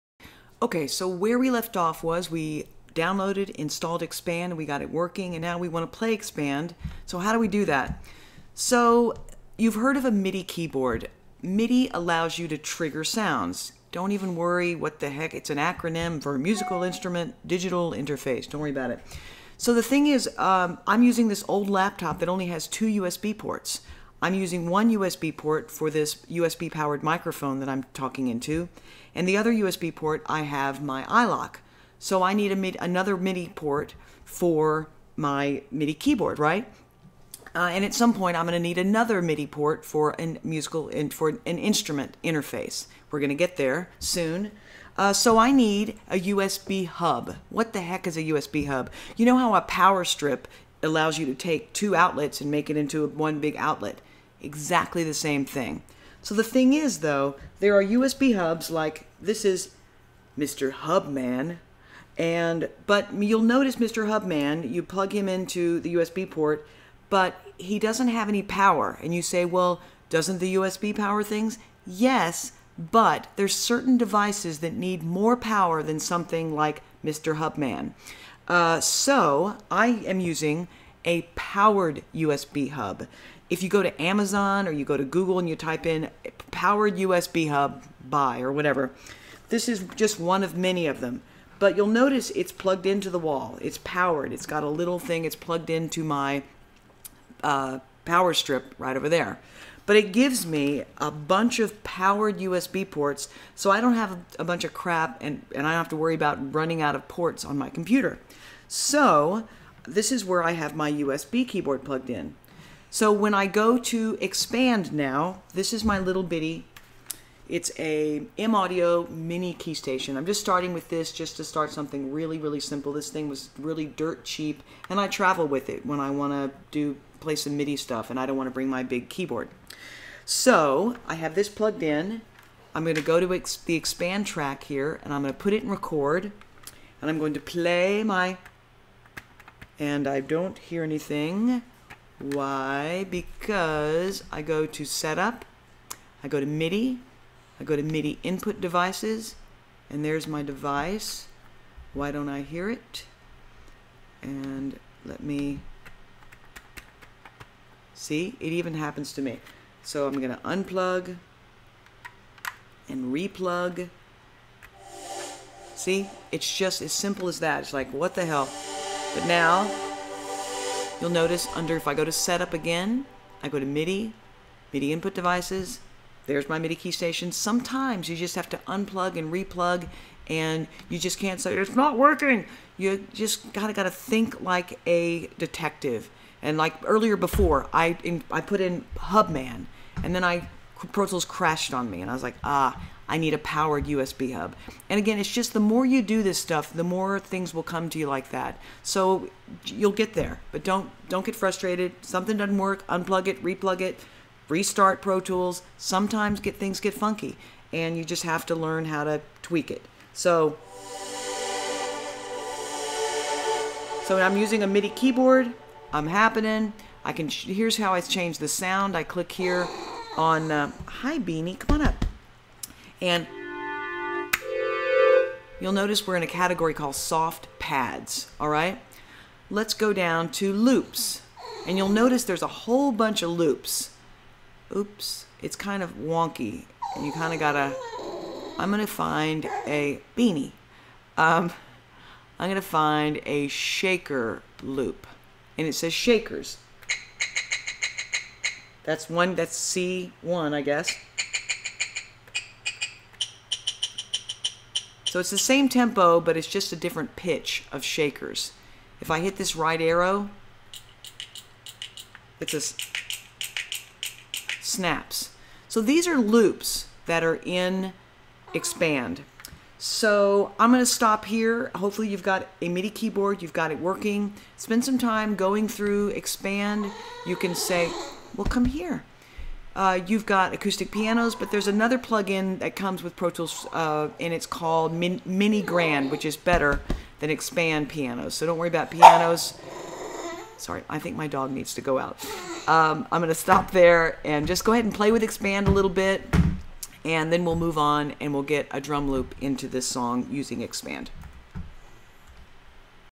Pro Tools in PJ's - Getting MIDI working
I connect a tiny MIDI keyboard and show you how to get MIDI working in Pro Tools.